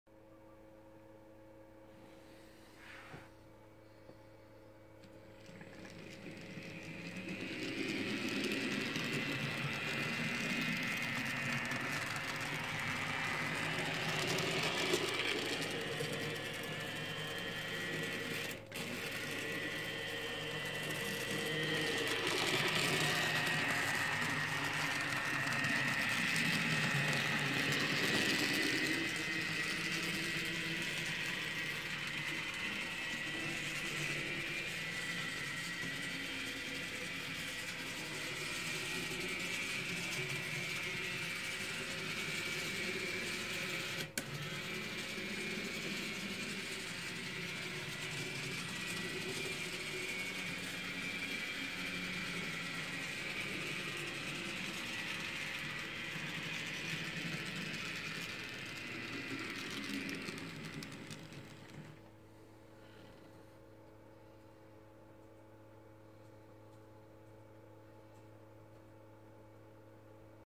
Ticket #176828
Graham Farish N Gauge Deltic been tested in DC after repair